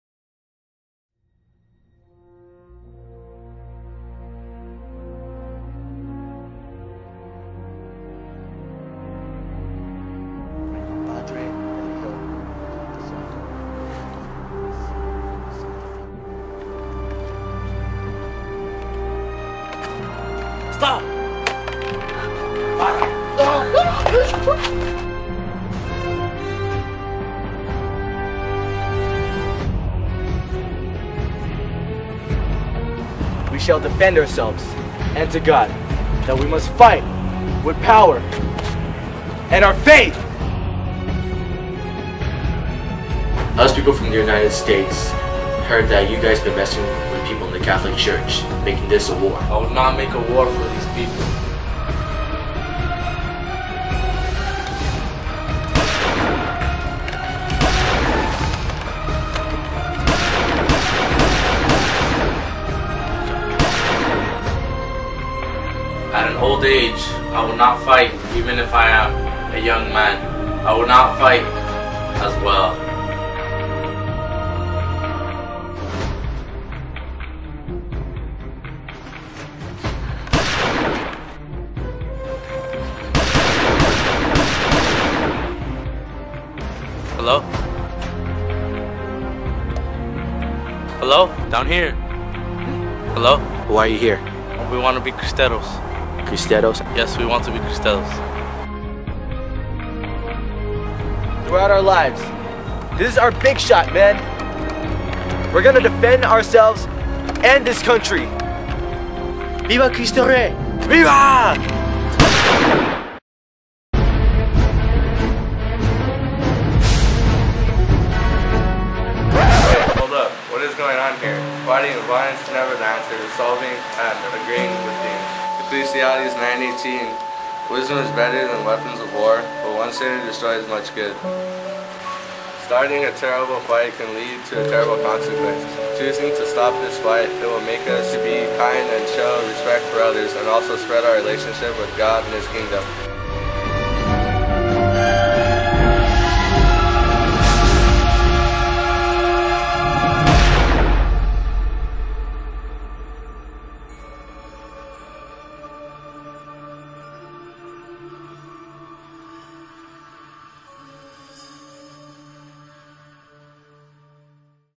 CRISTEROS TRAILER (CRISTIADA SPOOF) Thanksgiving Special
Hey Guys...
Sorry that there is once again no cwalk video..
This is for my project with my friends.
Very funny video for you guys, I hope you guys enjoy it.
Also, be thankful for what you have and what God has done for us!
HAPPY THANKSGIVING!
Laters, God Bless Everyone!